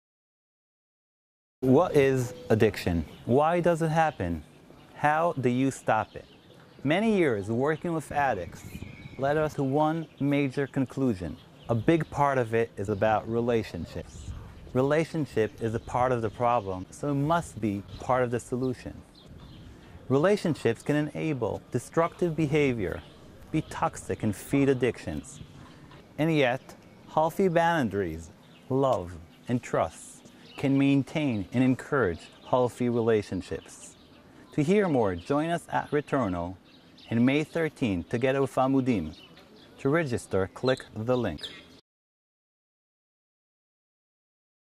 Retorno Amudim 2019 Codependence Event
Join Retorno and Amudim on May 13, 2019 to learn more about how relationships affect addiction.